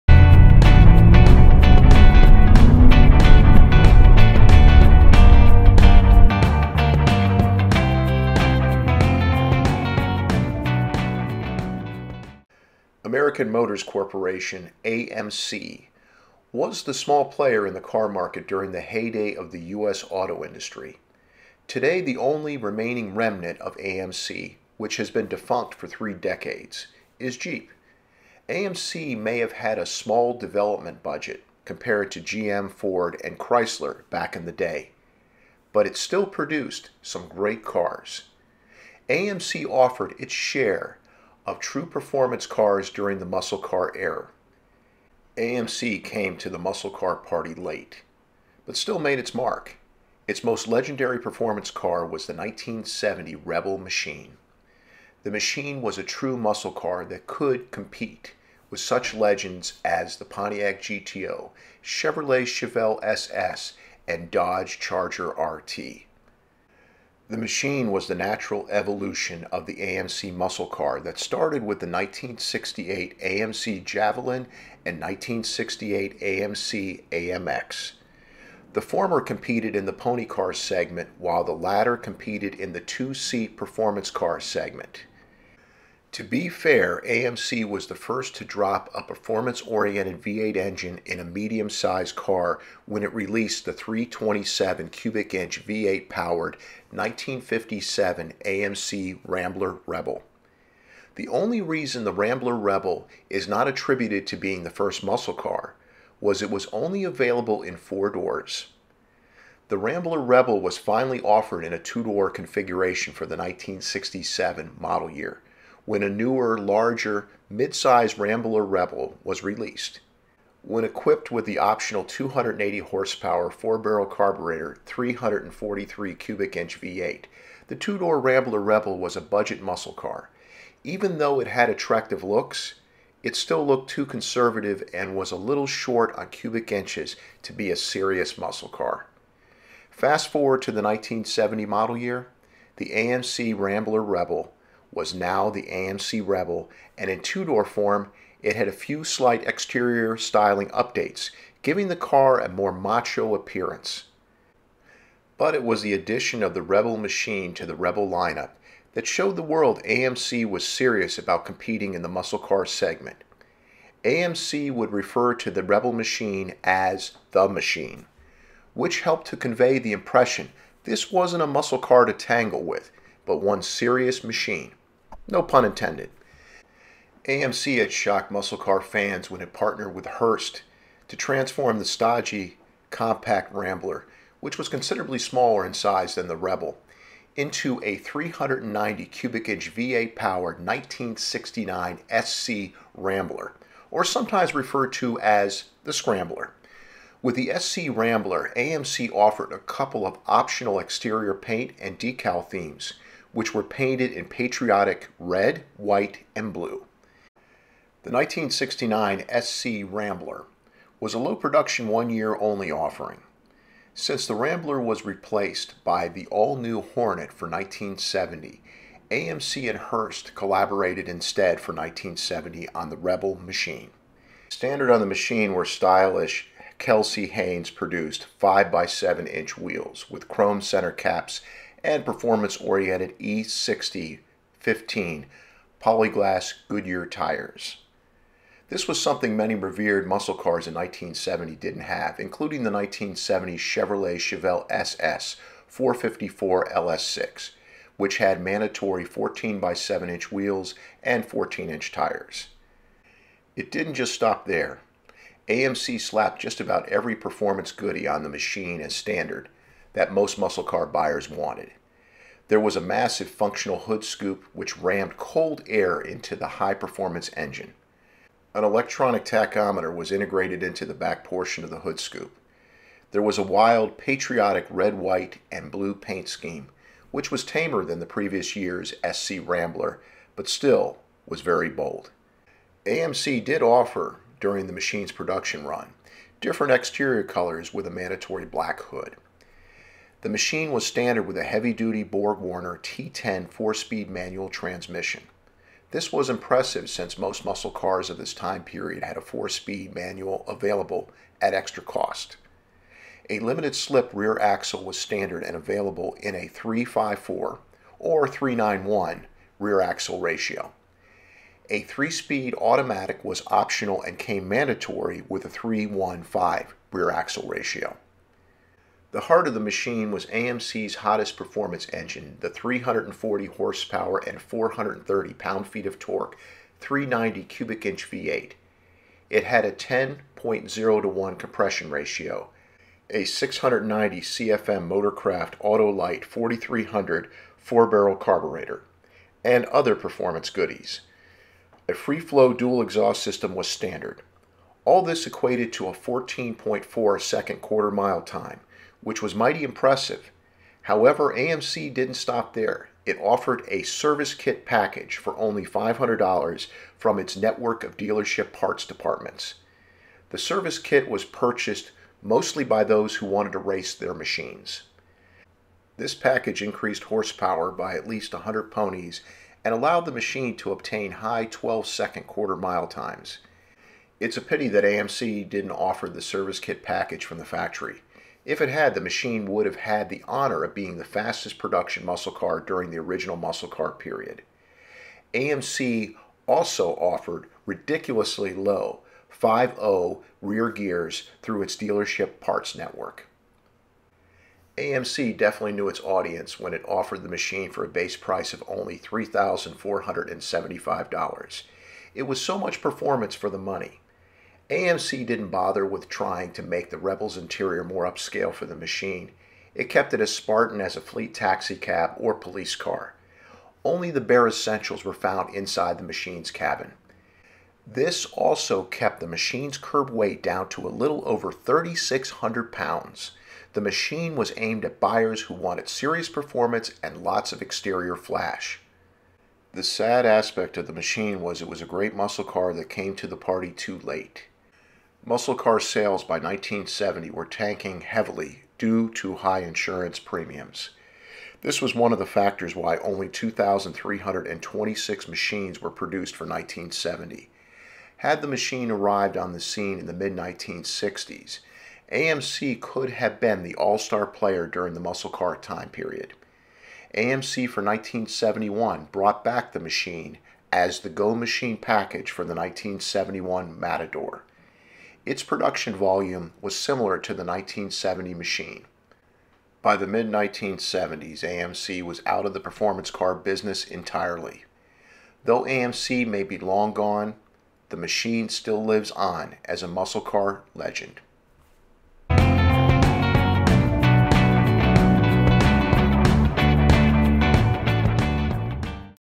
1970 AMC Rebel Machine - Don't Mess With The Machine!
AMC came late to the muscle car party, but it still made a formidable contribution with its share of solid muscle cars. The best was the 1970 Rebel Machine which provided just about every muscle car option as standard any gearhead could want. The Machine gave AMC legitimate muscle car credentials, and has become a bonafide muscle car legend. Here's the story behind AMC's best muscle car, and why you don't mess with The Machine.